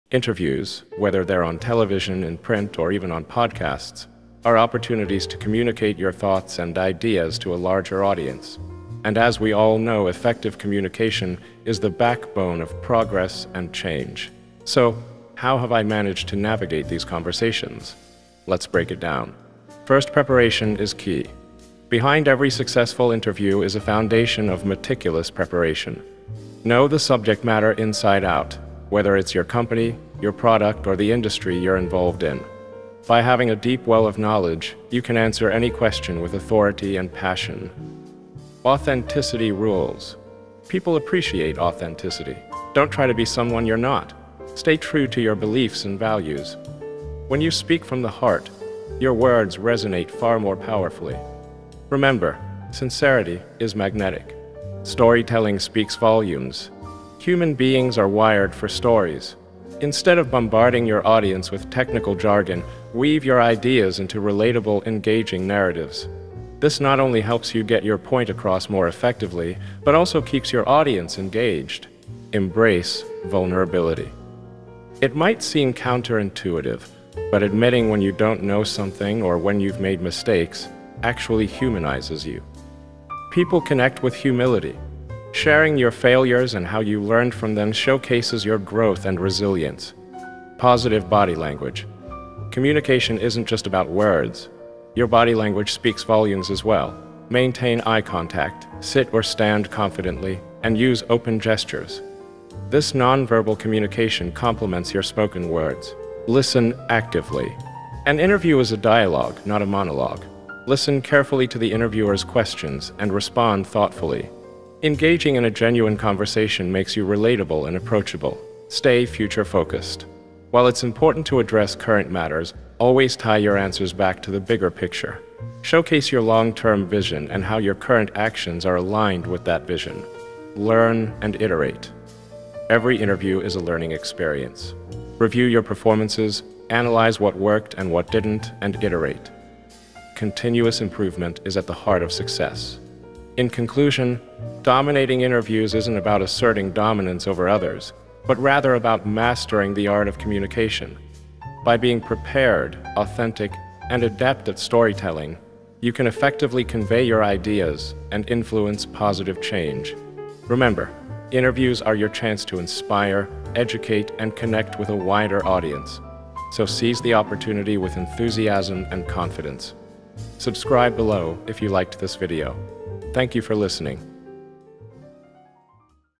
Elon Musk Interview Domination Tips
Some Words From Elon about how you can dominate any interview